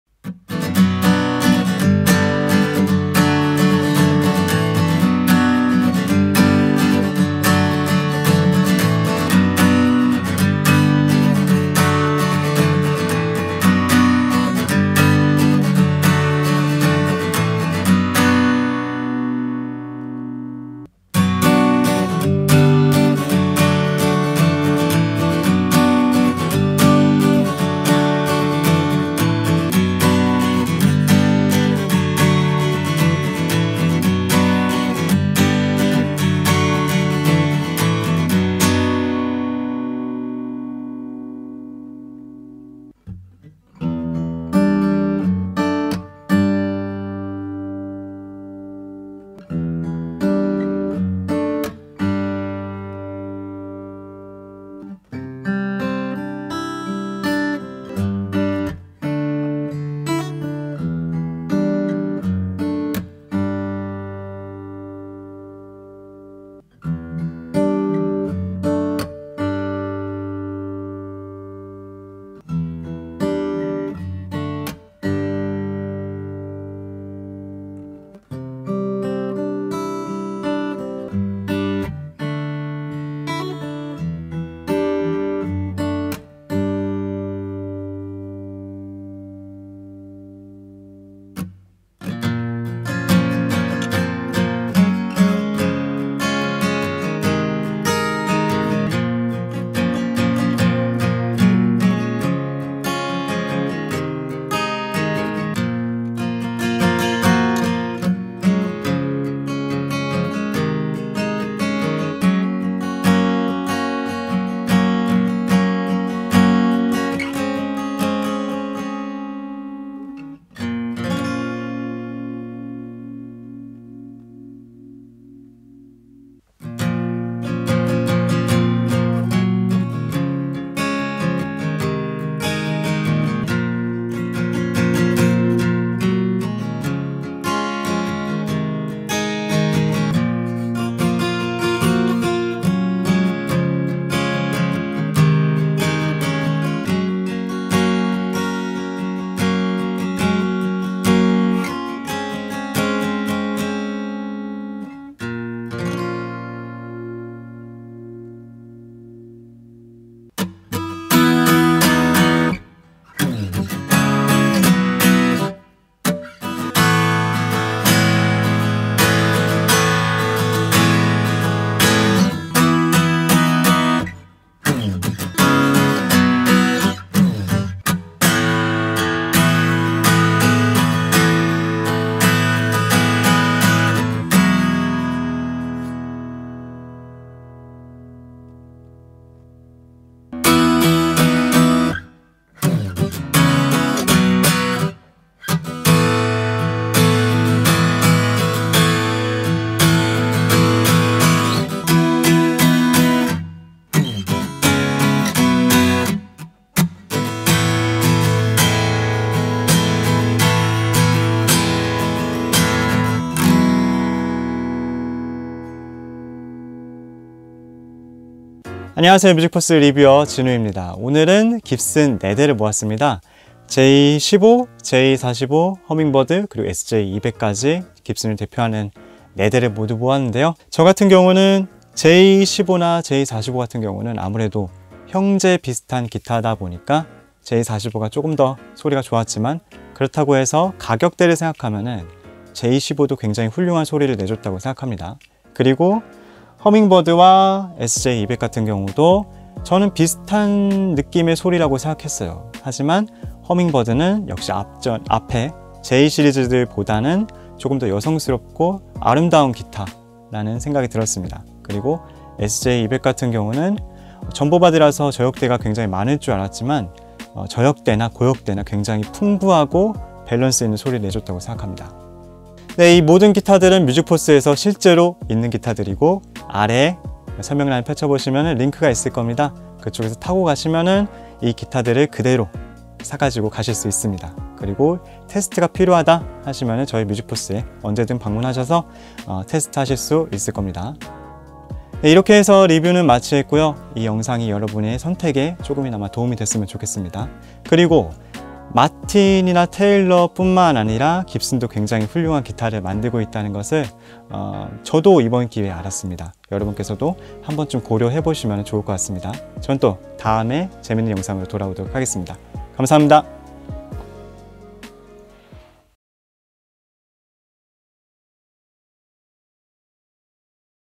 [Gibson Acoustic Review] SJ200 VS Hummingbird VS J45 VS J15
Music Force Gear Review Acoustic

Gibson Montana 2019 Model J-15 Walnut Burst w/L.R. Baggs Element Pickup System (MusicForce In Stock)

Gibson Montana 2019 Model J-45 Standard-Vintage Sunburst(신품) 깁슨 J45 스탠다드 (MusicForce In Stock)

Gibson Montana 2019 Model Hummingbird-Vintage Cherry Sunburst w/L.R. Baggs VTC Pickup System (MusicForce In Stock)

Gibson Montana 2019 Model SJ-200 Standard Antique Natural w/Gold HW(신품) 깁슨 SJ200 스탠다드(MusicForce In Stock)

Recorded by lewitt LCT940/ LCT140 Air Mic, UAD Apollo Twin mk2